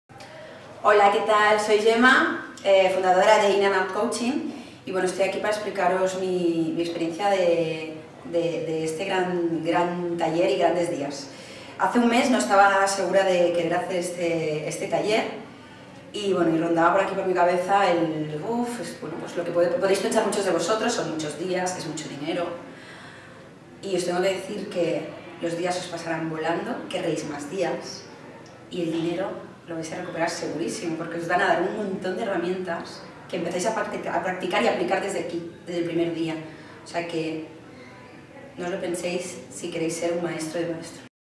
Maestro de Maestros-Gemma G Gasulla
Gemma es psicóloga, coach y formadora y participante de la 4 edición de Maestro de Maestros.
Nos cuenta su experiencia en un taller al que llegó llena de dudas y terminó encantada de haber estado. De hecho te invita a participar.

Si eres formador y quieres dar un salto en tu profesión tanto en diseño, impartición como venta de tus cursos. ¡Infórmate Ya!